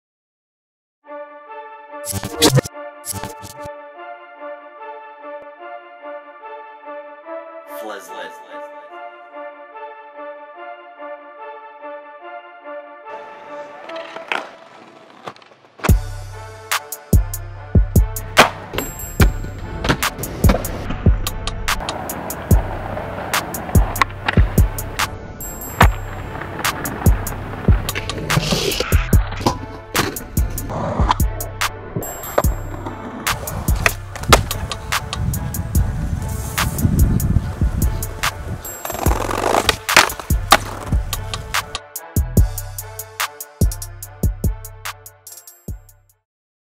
Teaser - Welcome Rafael Ramos
O paulistano Rafael Ramos é o mais novo integrante do skate team Aversion Outfits Co.
Confira o teaser de seu "Welcome Video" que será lançado dia 17/04/2017.

Fiquem ligados.